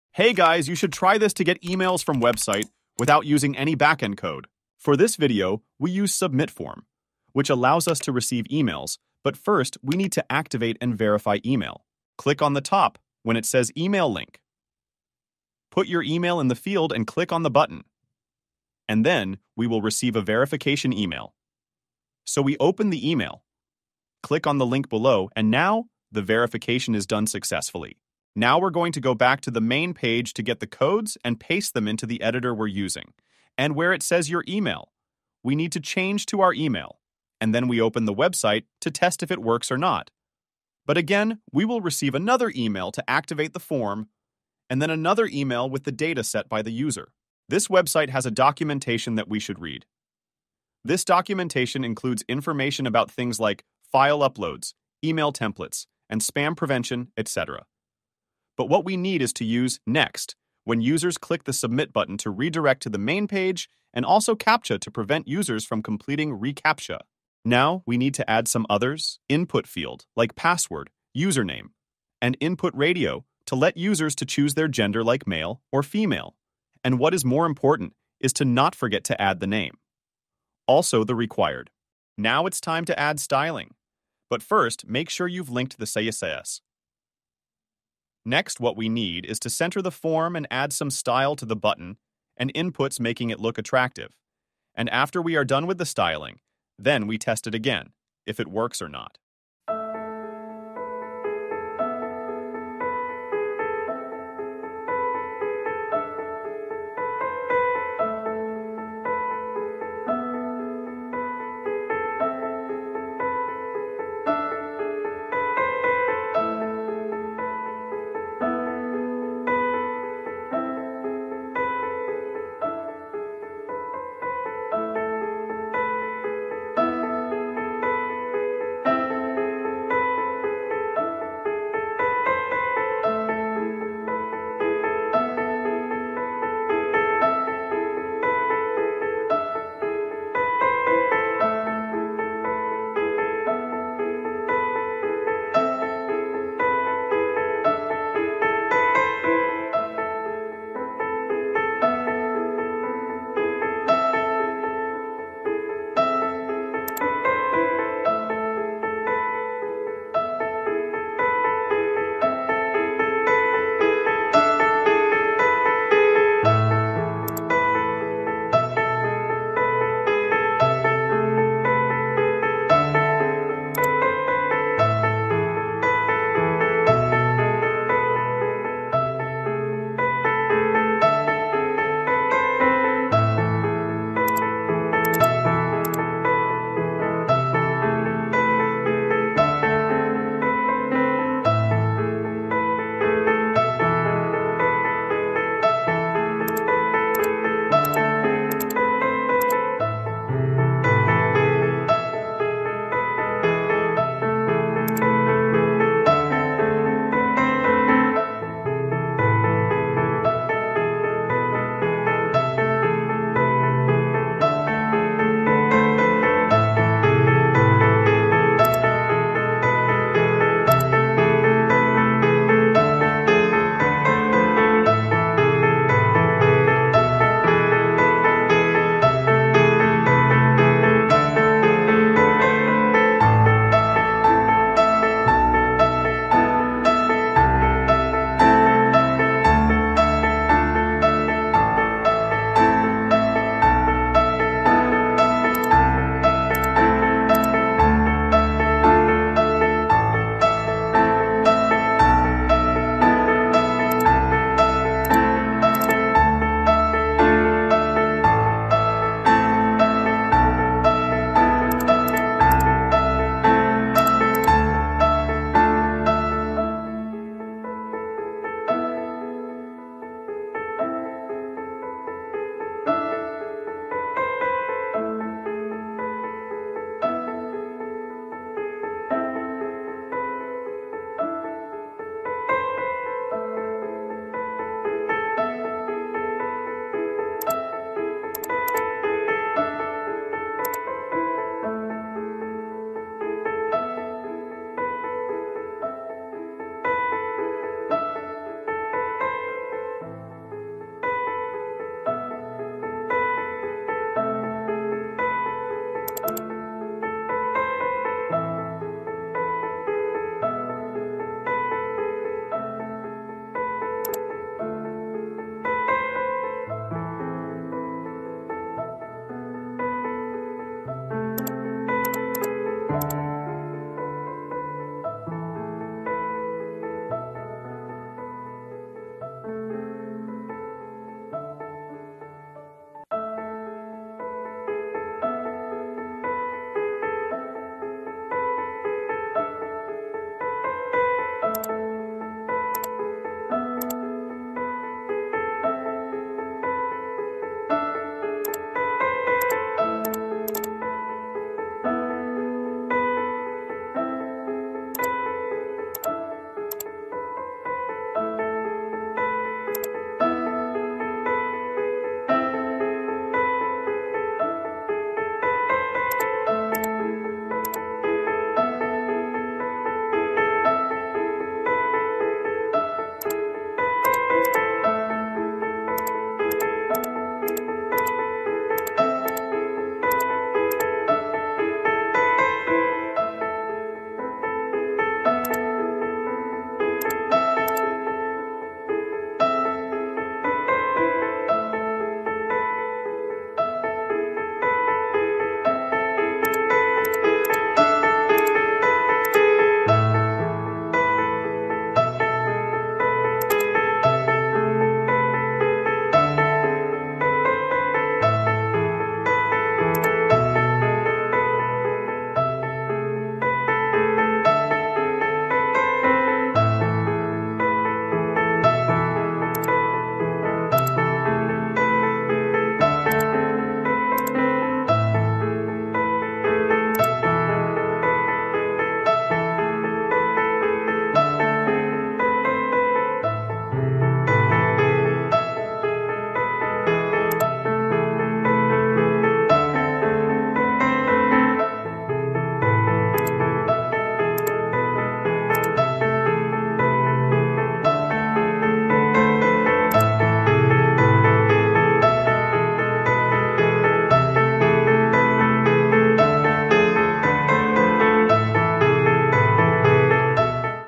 How To Send Email from HTML Contact Form Without Any Backend Language
In this video, I will show you how to create a simple contact form that sends emails directly to your inbox using only HTML . No need to learn any backend programming languages like PHP or Python!

0:06  --  Verify Email
0:29  --  Add codes to the editor
0:50  --  read the documentation
1:08  --  add some inputs (Password , radio etc )
1:22  --  Add some style